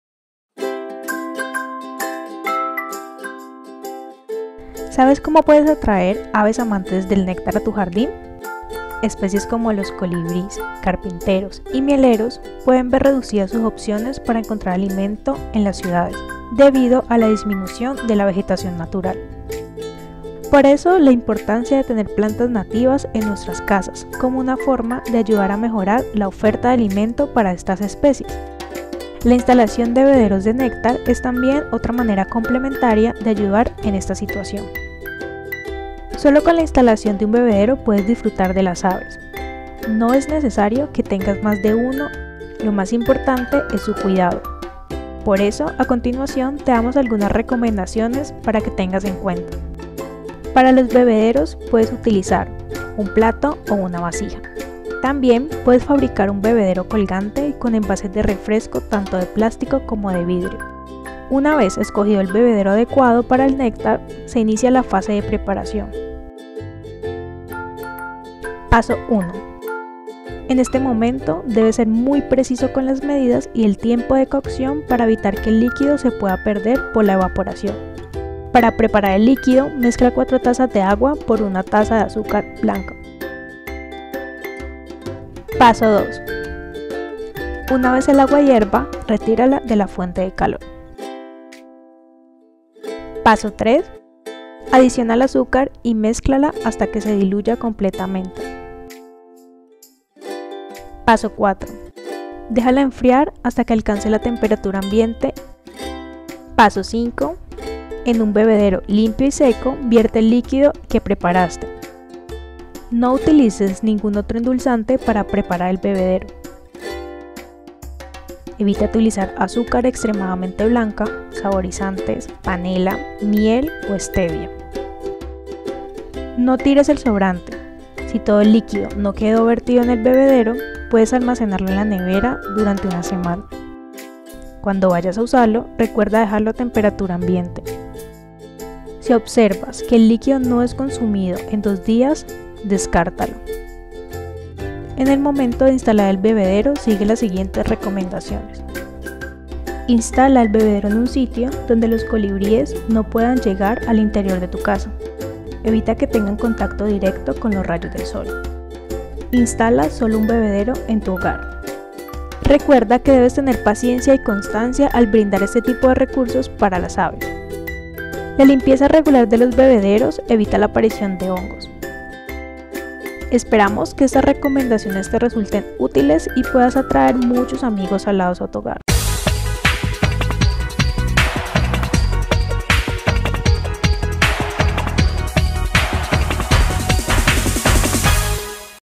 Bebederos para las aves
Si te gusta estar rodeado de colores y alas, una de las cosas que debes saber es que si instalas de manera responsable un bebedero, puedes atraer a las aves amantes del néctar a tu vivienda o jardín. En este vídeo te explicamos la forma correcta de hacerlo para que puedas disfrutar de su compañía.

Si deseas conocer más sobre este tema, te invitamos a visitar nuestra página web 🌐 y descargar la publicación Un buen amigo de las aves en los espacios urbanos